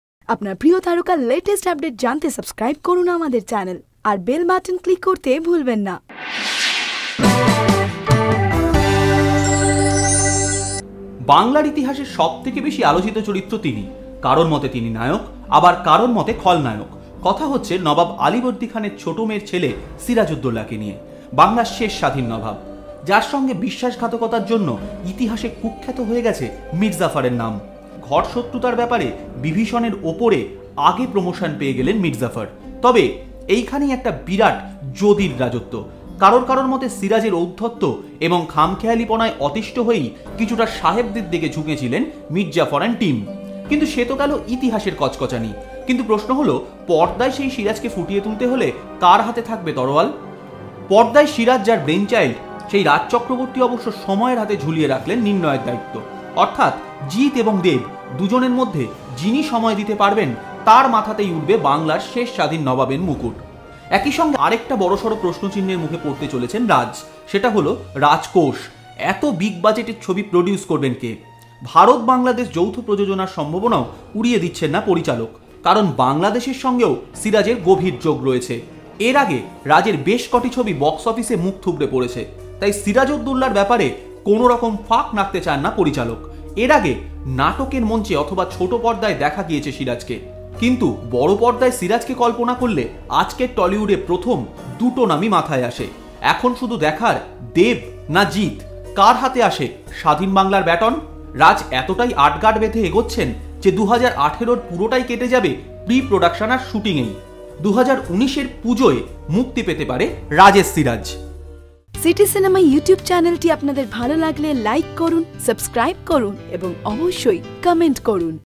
Dev না Jeet সিরাজ কে ?
He is one of the most controvertial character in bengal history. For few he is a hero, but at opposite side for few persons he is a negative character. We are talking about the grandson of nawab Alibardi khan, nawab Siraj-ud-daulla.